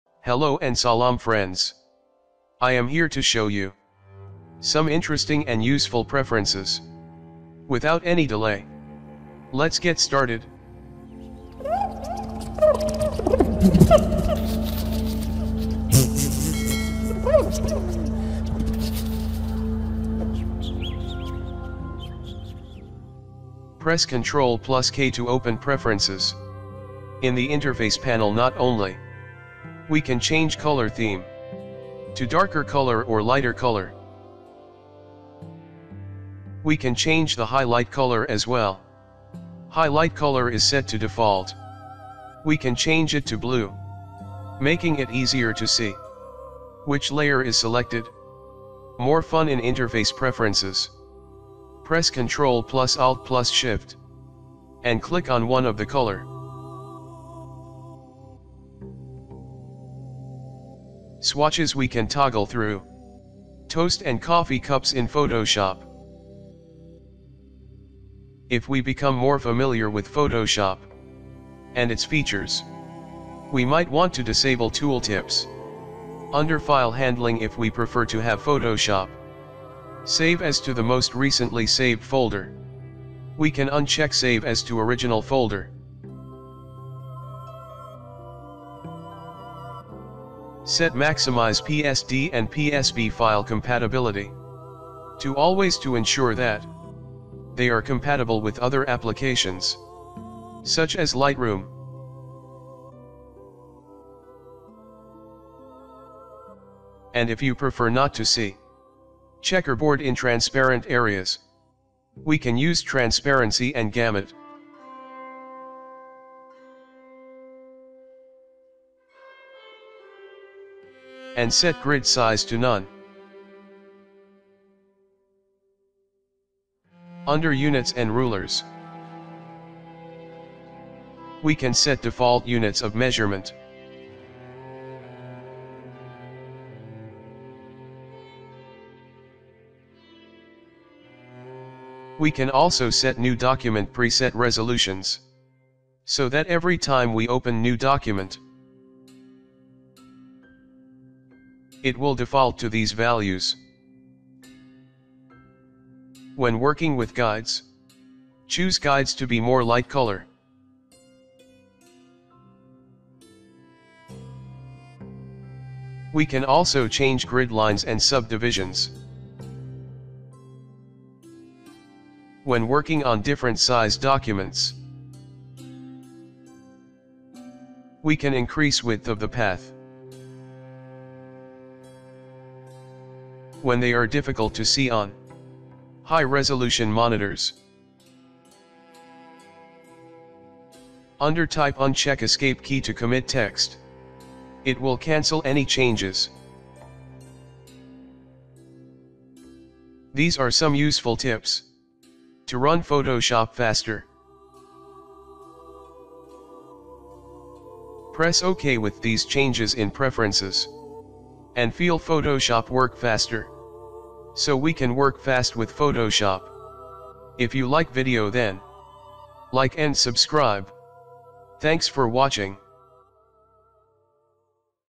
Photoshop Useful Preferences
Learn how to improve Photoshop's performance, customize the interface, save backups of your work, and more with the important options you need to know about in the Photoshop Preferences. Break down every option and secret of tool has to offer so that you can choose the one that works best for you.